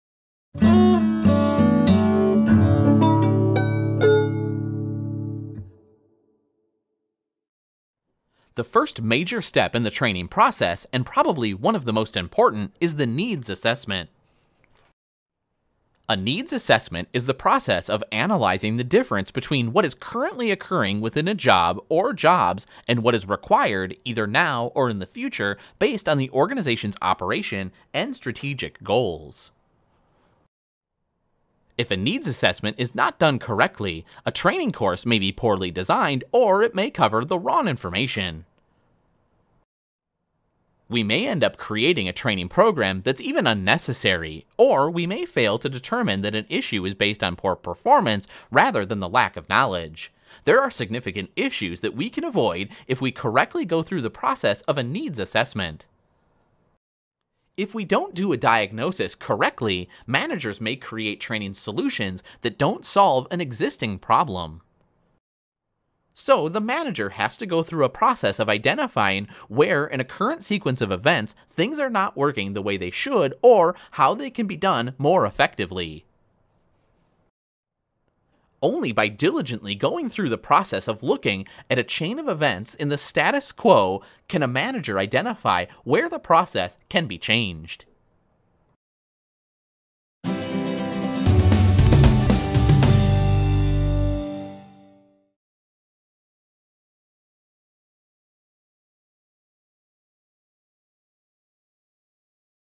Needs Assessment
The first major step in the training process, and probably one of the most important, is the needs assessment. A needs assessment is the process of analyzing the difference between what is currently occurring within a job or jobs and what is required—either now or in the future—based on the organization’s operations and strategic goals. If a needs assessment is not done correctly, a training course may be poorly designed, or it may cover the wrong information. The wrong employees may be asked to participate in the training, or they may not yet be capable of absorbing the information in the training because of a lack of a knowledge base or skill set.

We may end up creating a training program that’s unnecessary, or we may fail to determine that an issue is based on poor performance rather than lack of knowledge. These are significant issues that we can avoid if we correctly go through the process of a needs assessment.

Similar to those good physicians and automobile mechanics that we just mentioned, organizational managers have to diagnose what may currently be wrong with a process so that they can successfully repair and/or tune the process up. If they don’t do the diagnosis correctly, managers may create training solutions that don’t solve the existing problem. So the manager has to go through a process of identifying where in a current sequence of events things are not working the way they should, or how they can be done more efficiently.

Only by diligently going through the process of looking at that chain of events in the status quo can a manager identify where the process can be changed to improve organizational productivity and reach the organization’s goals.